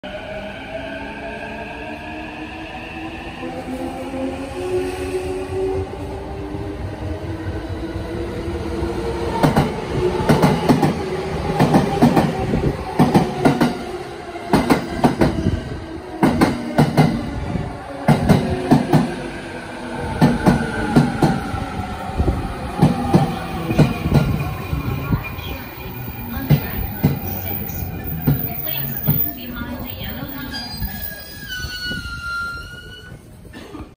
【ジョイント音立てて入線】東海道線 茅ヶ崎駅
茅ヶ崎駅のジョイントは上りのほうが有名ですが下りにもあります